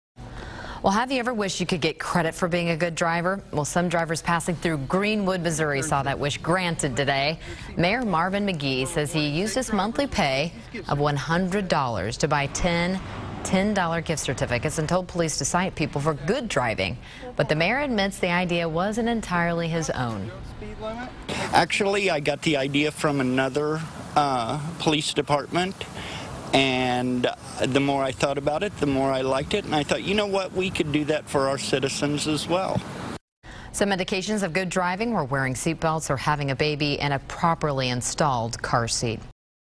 Mayor gives tickets for good driving
Some drivers in Greenwood, Mo. were surprised to get tickets for good driving Saturday.